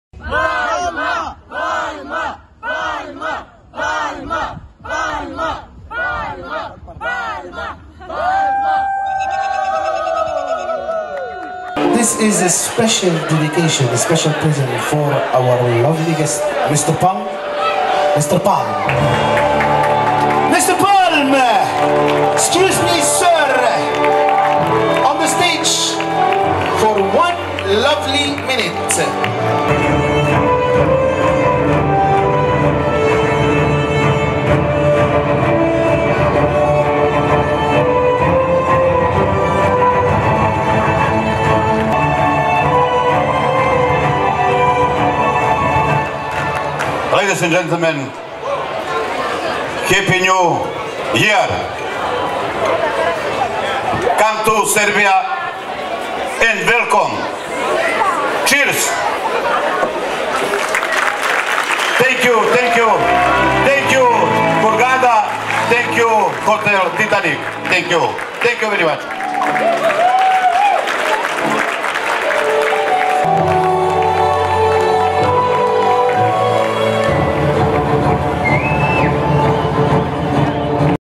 Gde god se pojavi, Palma pokupi simpatije
Dragan Marković Palma u poseti Egiptu